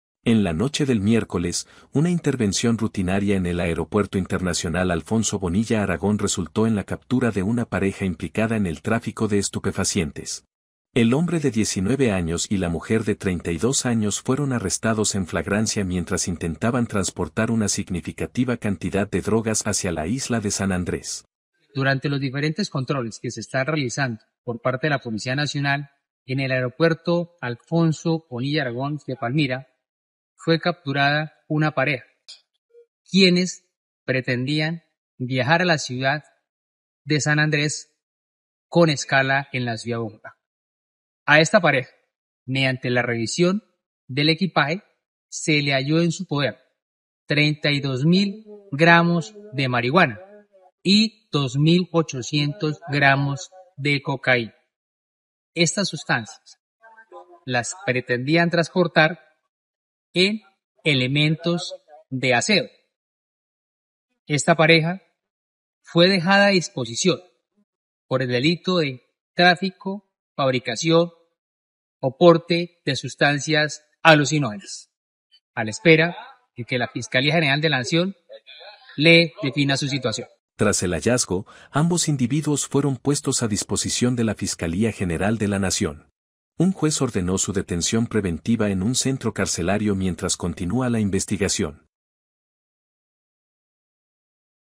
Pareja detenida con 34 kilogramos de drogas en el Aeropuerto Internacional Alfonso Bonilla Aragón.
Una operación rutinaria de inspección de equipaje llevada a cabo en la noche del miércoles, autoridades del aeropuerto internacional Alfonso Bonilla Aragón descubrieron una considerable cantidad de estupefacientes escondidos entre ropa y productos de aseo personal. La pareja implicada, un hombre de 19 años y una mujer de 32 años, fue capturada en flagrancia por el delito de tráfico, fabricación o porte de estupefacientes.